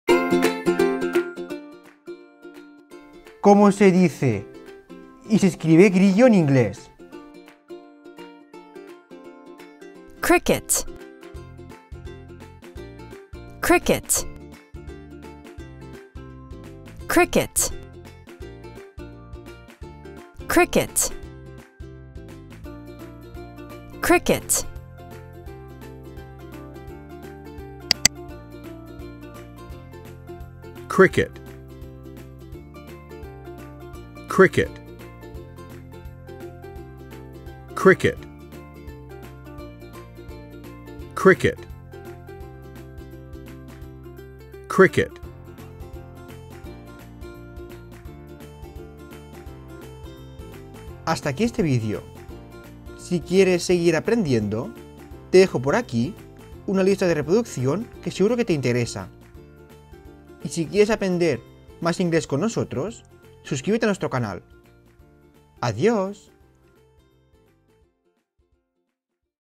✅CÓMO se dice GRILLO en INGLÉS 2022 🚀 APRENDE A ESCRIBIR LIME ✨ PRONUNCIACIÓN CRICKET EN INGLES 2023
▶️ Aprende a CÓMO decir y escribir GRILLO correctamente en inglés ✅

Introducción: 00:00
Pronunciación en femenino: 00:08
Suscríbete: 00:25
Pronunciación en masculino: 00:29
Lista de reproducción recomendada: 00:49